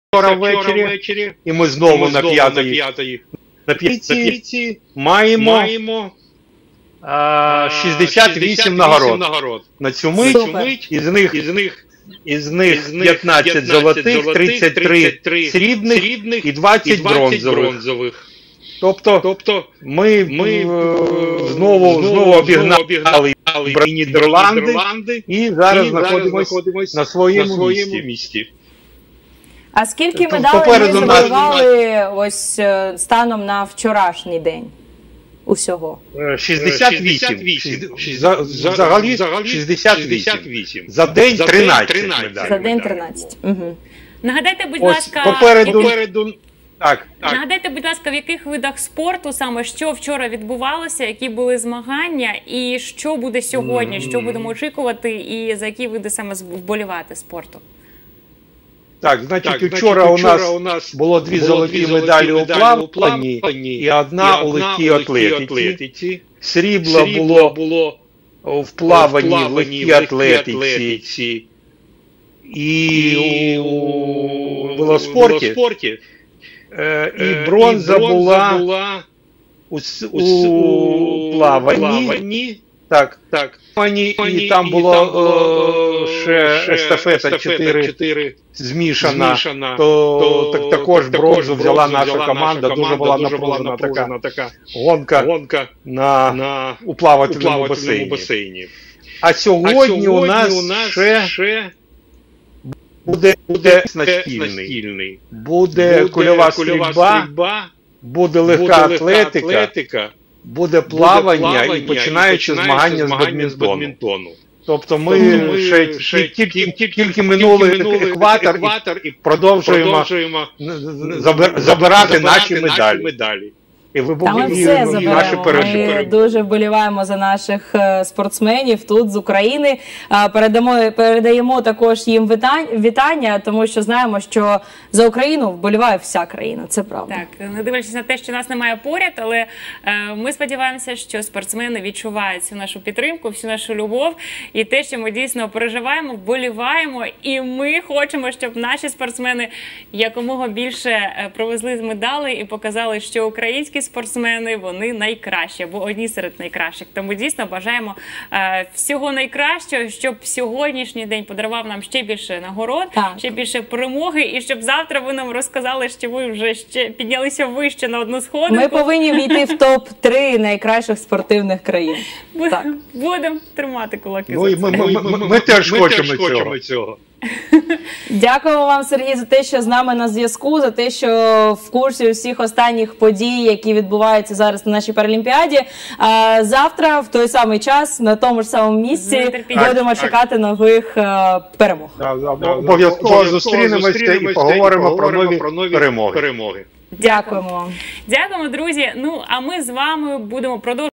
Пряме включення з Токіо. 8-й день Паралімпіади
Україну представляють 143 атлети, які змагаються у 15 видах спорту. Всього ж в іграх представлено 22 види та беруть участь 168 країн.  У Токіо наразі працюють наші колеги, тож маємо унікальну можливість дізнаватися про все першими.  Останні подробиці з паралімпіади нам вже готовий розповісти Сергій Педашенко